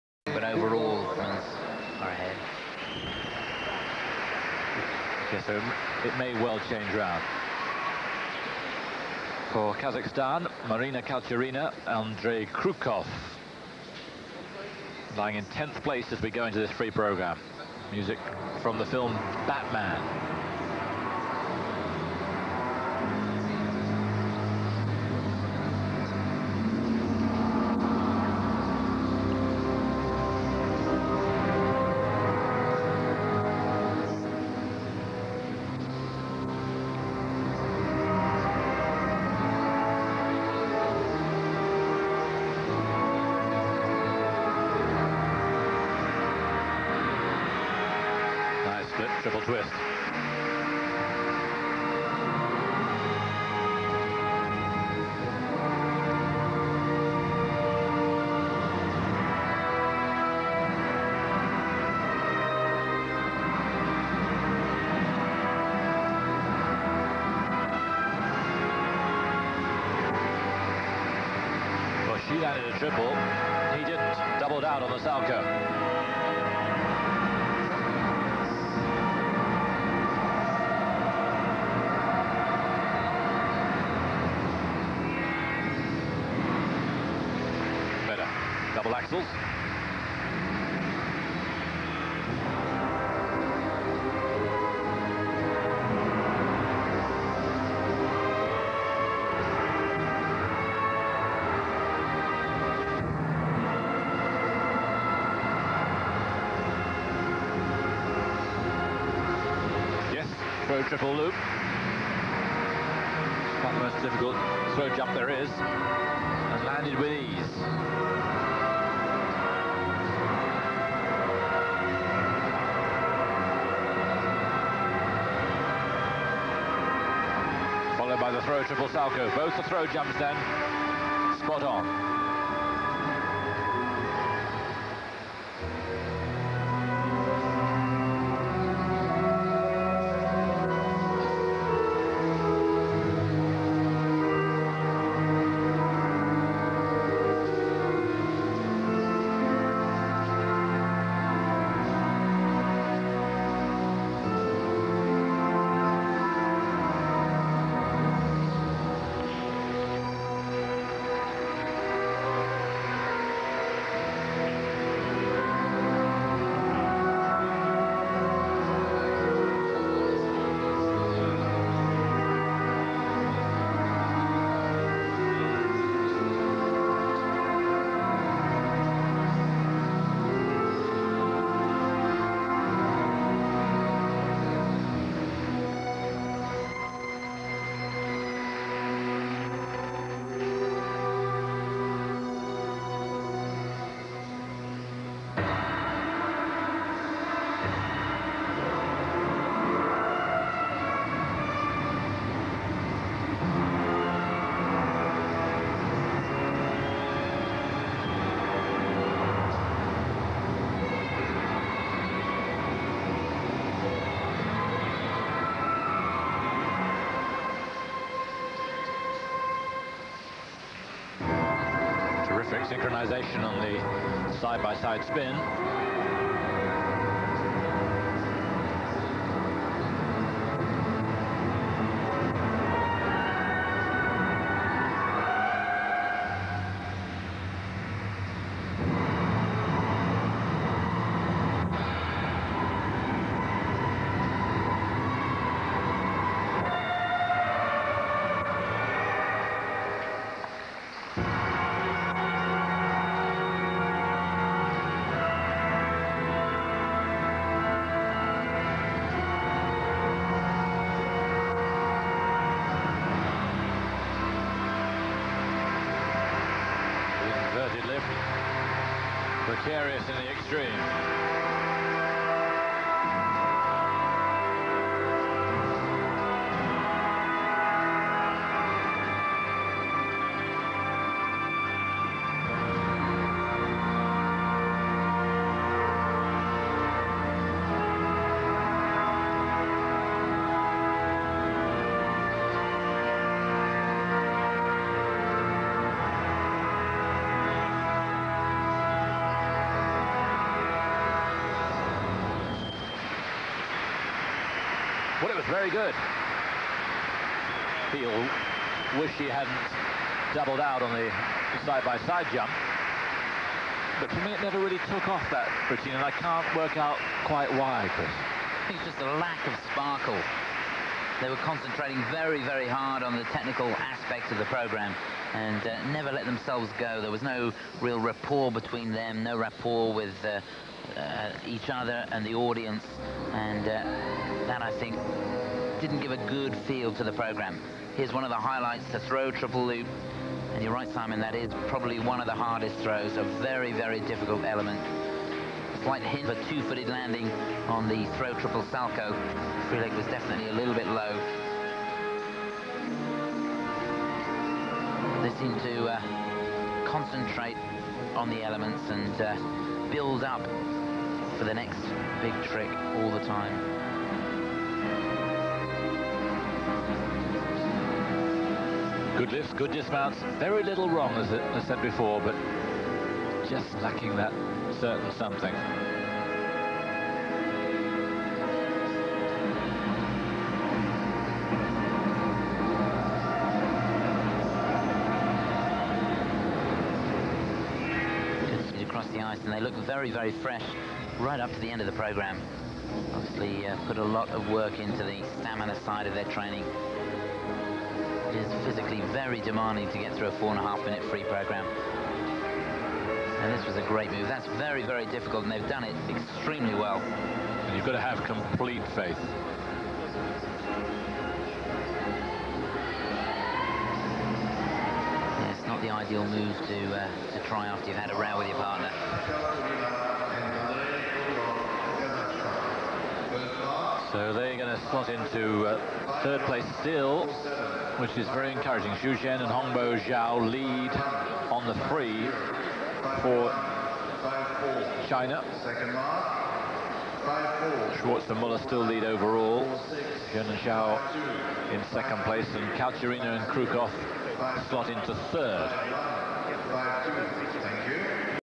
W1997 Pairs LP Mariana Khalturina Andrei Khrukov KZK
1997 World Figure Skating Championships, Lausanne 1997
British Eurosport (I do not own this material)

As with their short program, a lot of technical difficulty and innovation to admire; even by doing side-by-side salchows was refreshing in this era of pairs skating!  Their biggest challenge was the music choice, it somehow became 'lost' in the performance, and it was difficult as a viewer to connect with it.

11th in LP standings; 12th overall